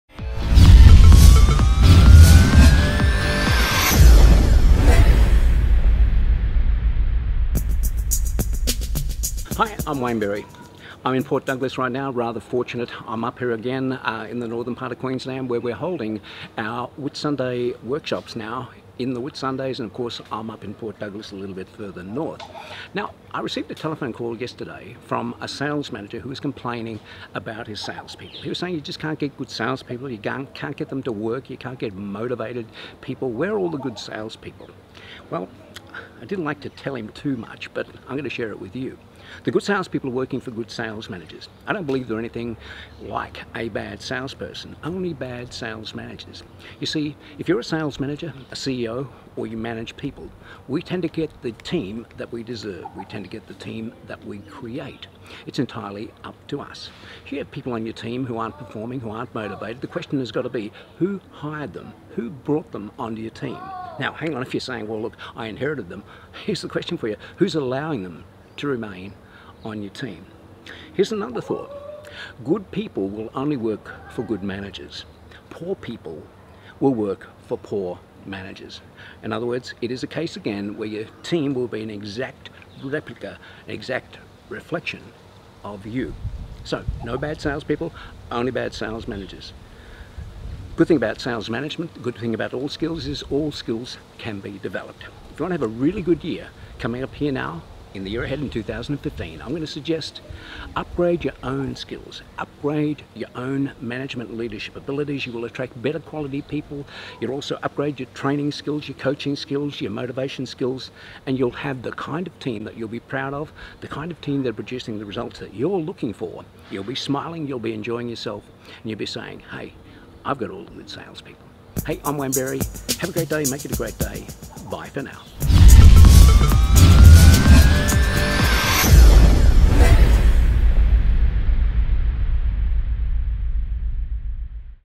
There are no bad sales people, only bad sales managers
Wayne Berry explains why it is every sales manager's responsibility to create a team of successful sales people.  If they fail to do this, they are failing as a sales manager.  This is one of Wayne Berry Weekly Top Gun Video Sales Tips.